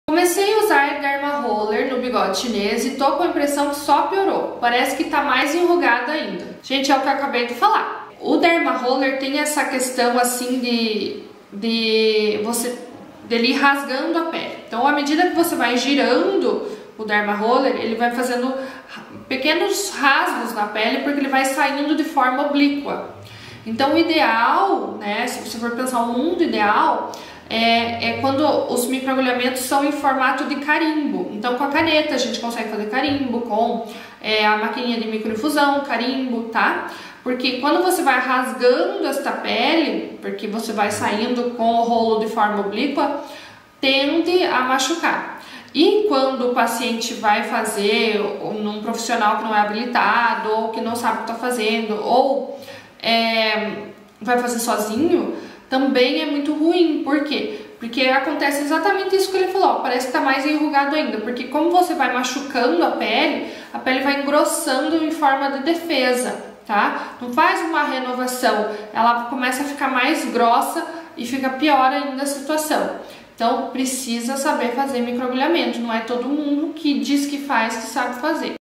Quer colher bons resultados com microagulhamento?
Eu amo os resultados do microagulhamento, ainda mais quando associado à drug delivery.
Porém, precisamos estar sempre atentas nos movimentos do dermarroler ou usar instrumentos que façam perfurações tipo carimbo na pele, para que seja possível uma verdadeira renovação celular e não um engrossamento da pele em forma de defesa.
Faça a Melhor HOF no seu paciente e entregue os melhores resultados do microagulhamento.

Se você gostou, acompanhe meus conteúdos para ficar por dentro das técnicas que lotam a sua agenda com A Melhor HOF para o seu paciente!